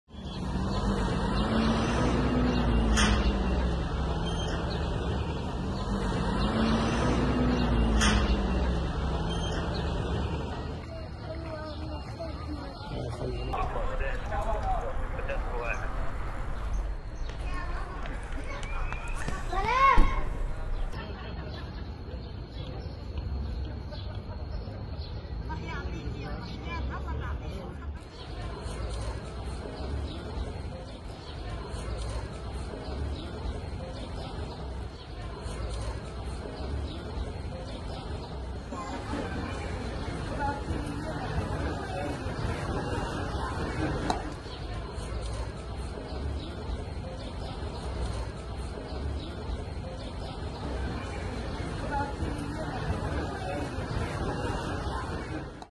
مشاهد عامة لمنطقة باب العامود بالقدس المحتلة
General views of Bab al-Amoud area in occupied Jerusalem.